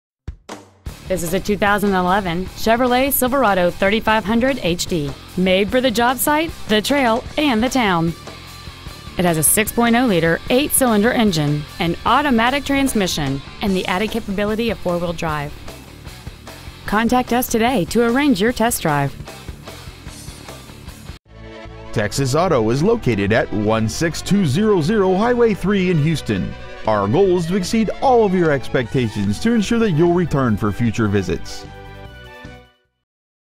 2011 Chevrolet SILVERADO 3500HD Houston TX
Year : 2011
 Make : Chevrolet
 Model : SILVERADO 3500HD
 Engine : 6.0L Vortec 1000 VVT V8 SFI Engine
 Trans . : Automatic
 Exterior : Black
 Miles : 71,291
 Interior : Black

 Preowned 2011 Chevrolet SILVERADO 3500HD Houston TX